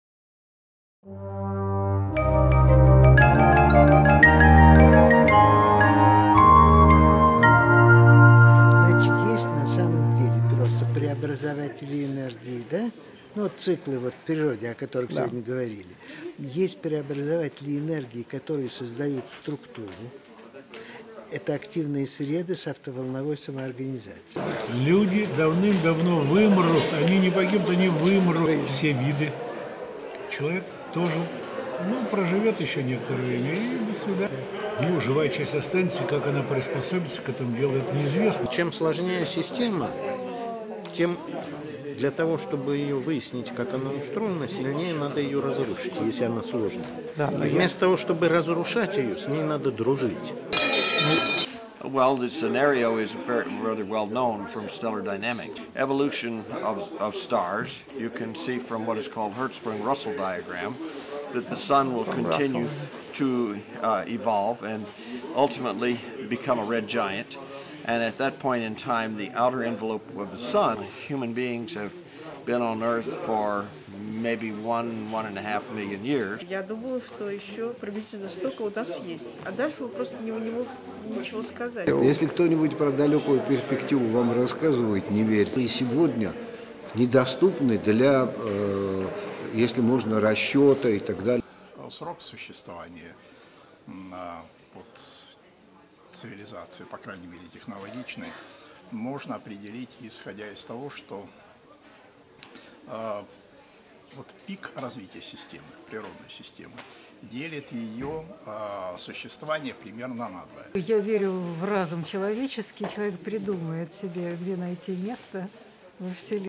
Астробиология 2016 (анонс)
2-я Всероссийская конференция по астробиологии (Анонс).
Жизнь во Вселенной: Физические, химические и биологические аспекты. 2016 г.